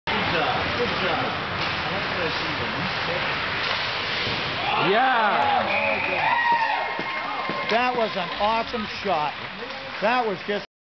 Shanny Snipe
Hockey Game Vs. NOTL Shanny Scores Once Again!!!